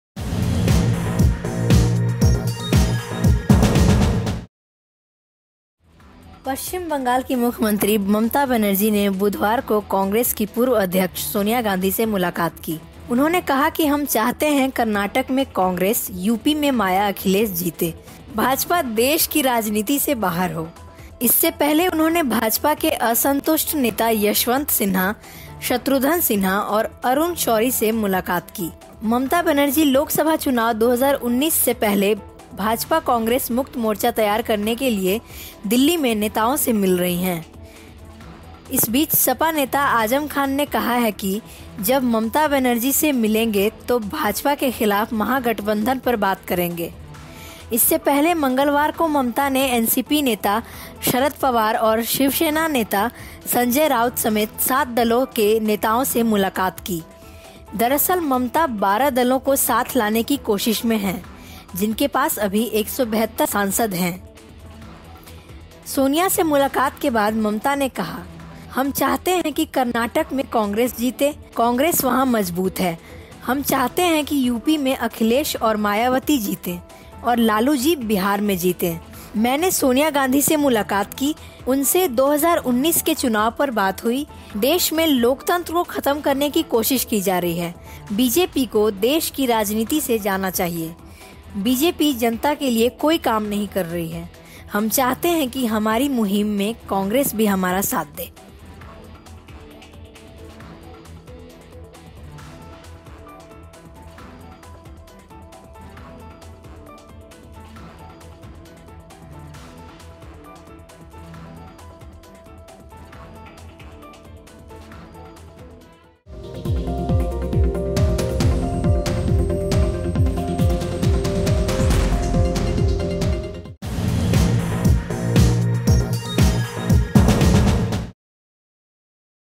यूपी में माया-अखिलेश, कर्नाटक में कांग्रेस जीतें-ममता बनर्जी | Media Reports
पश्चिम बंगाल की मुख्यमंत्री ममता बनर्जी ने बुधवार को कांग्रेस की पूर्व अध्यक्ष सोनिया गांधी से मुलाकात की। उन्होंने कहा कि हम चाहते हैं कर्नाटक में कांग्रेस, यूपी में माया-अखिलेश जीतें। भाजपा देश की राजनीति से बाहर हो। इससे पहले उन्होंने भाजपा के असंतुष्ट नेता यशवंत सिन्हा, शत्रुघ्न सिन्हा और अरुण शौरी से मुलाकात की।